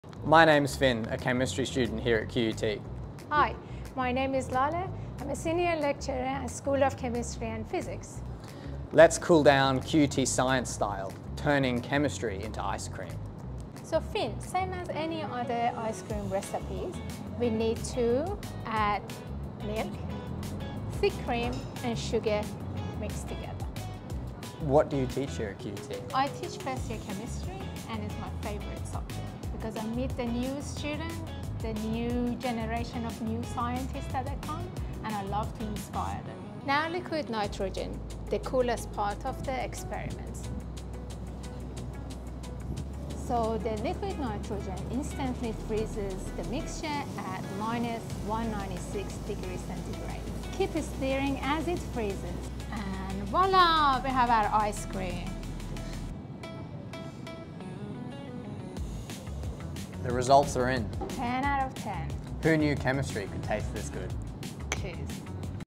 Let's Make Liquid Nitrogen Ice Cream!
What happens when you combine milk, cream, sugar, and chemistry? You get liquid nitrogen ice cream! Join chemistry student Fynn and senior lecturer Laleh as they beat the heat with the coolest science dessert ever 🍦☀️